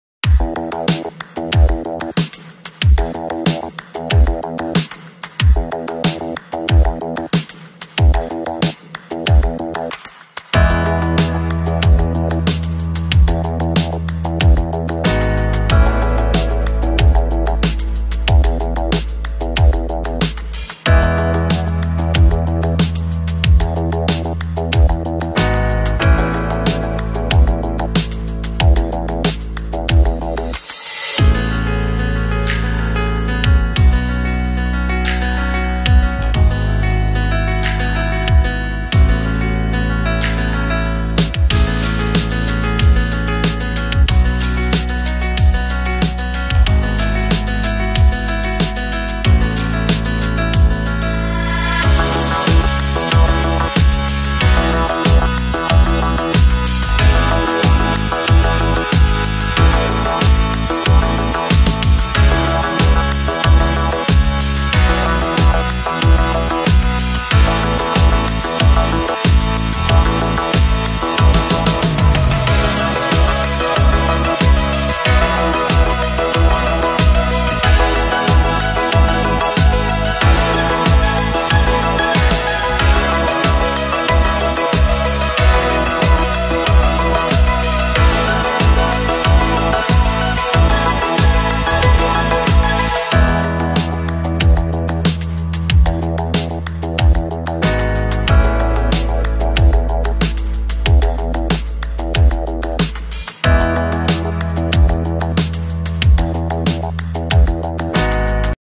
SHOULDERS WORKOUT AT HOME USING DIY GYM EQUIPMENTS / ARNOLD PRESS / DUMBBELL REAR DELTOID RAISE ...
Shoulders are small part in the body, therefore we can use light gym equipments to grow and shape it.